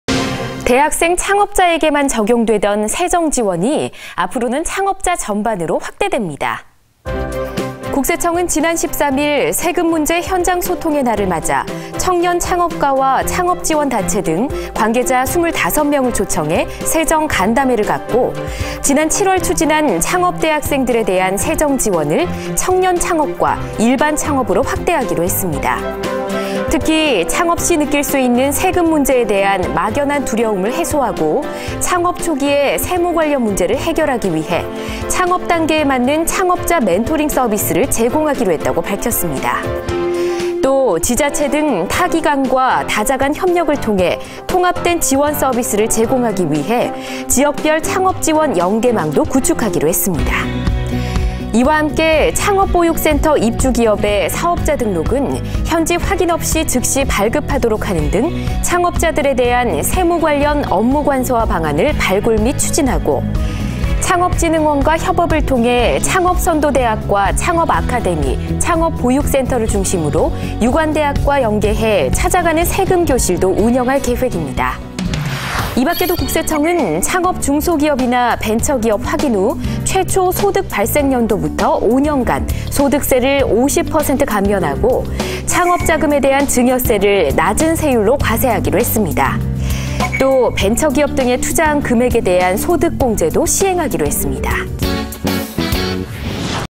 창업대학생에 대한 세정지원, 창업자 전반으로 확대
대학생 창업자에게만 적용되던 세정지원이 앞으로는 창업자 전반으로 확대됩니다.